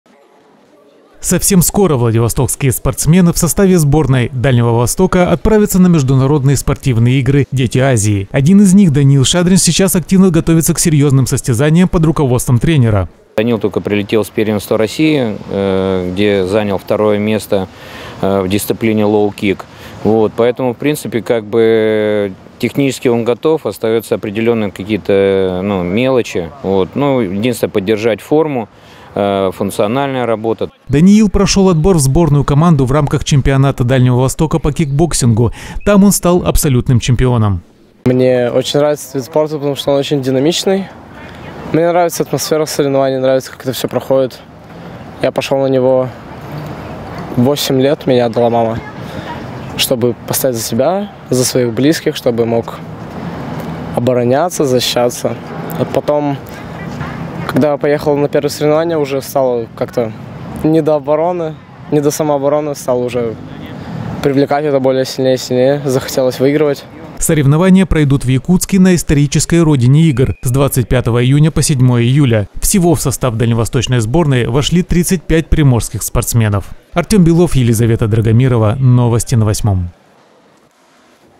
Владивостокский кикбоксер Даниил Шадрин активно готовится к международным играм «Дети Азии»
Совсем скоро владивостокские спортсмены в составе сборной Дальнего Востока отправятся на международные спортивные игры «Дети Азии». Один из них – Даниил Шадрин. Сейчас он активно готовится к серьезным состязаниям под руководством тренера.